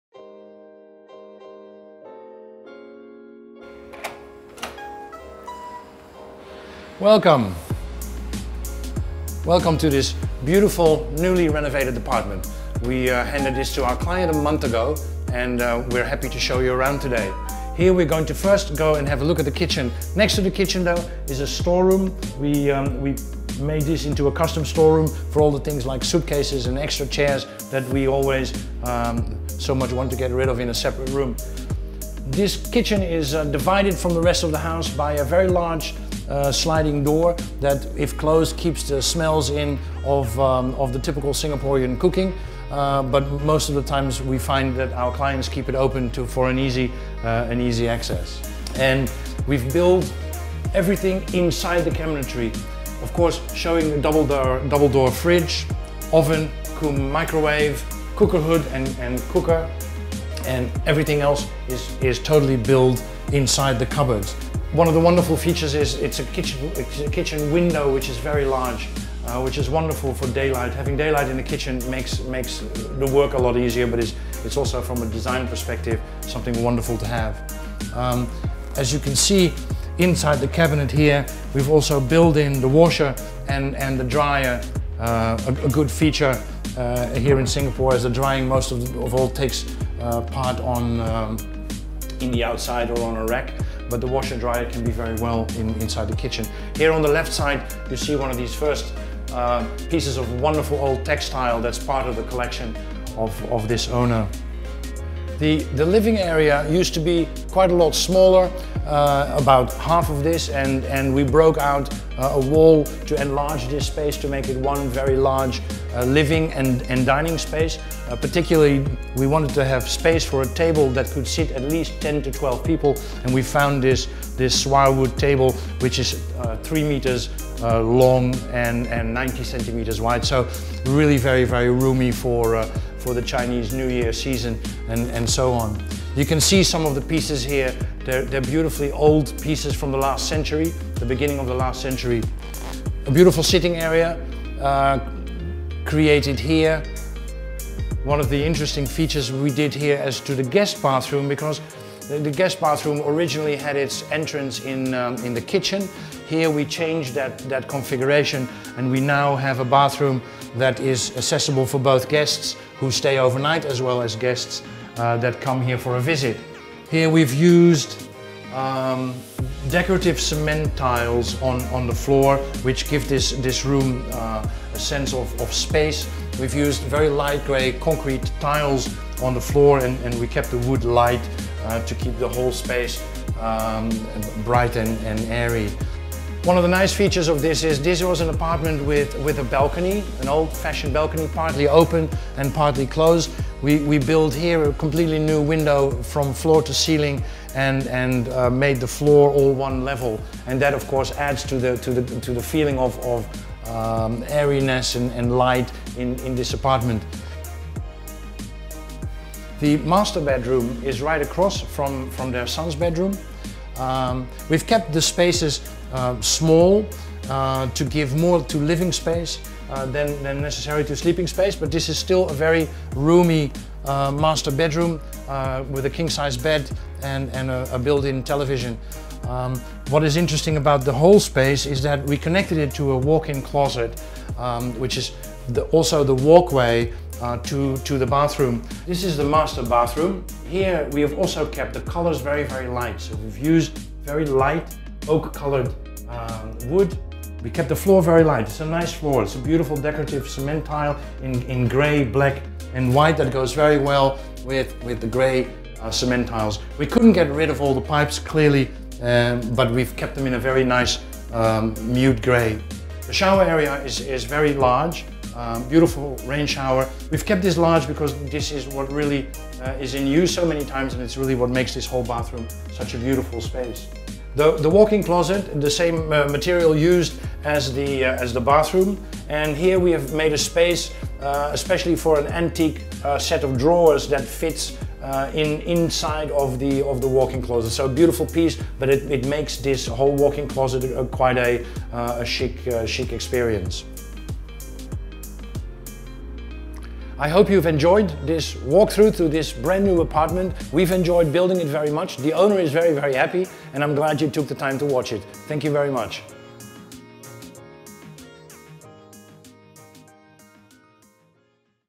House Tour: A modern five-room HDB home designed with antique accents
Singapore-based interior design studio ICIA designed a contemporary 5-room HDB apartment home for this Singaporean former banker and his family.